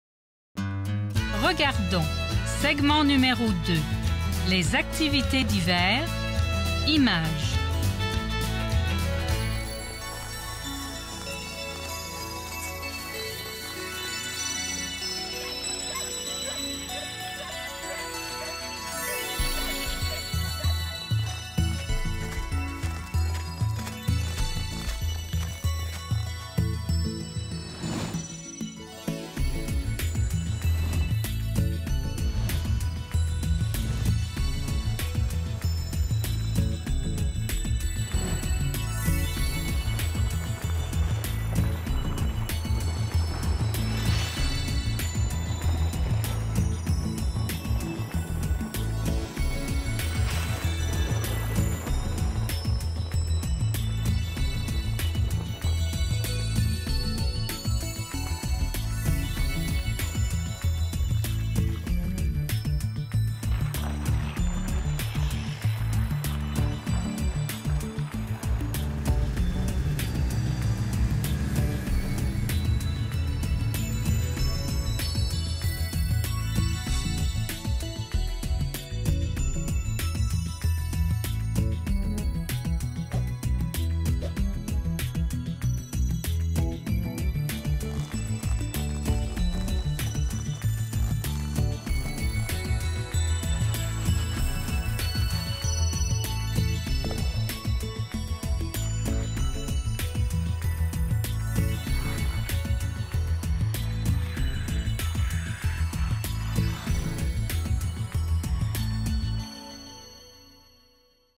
WINTER_CELEBRATIONS_2 Segment 2.m4v
Cool animation with music by Dwayne Ford. Part of the "French As A Second Language" videos. This product is an original creation of Alberta Education-Government of Alberta. The images used to create the clips are under license from Animation Factory.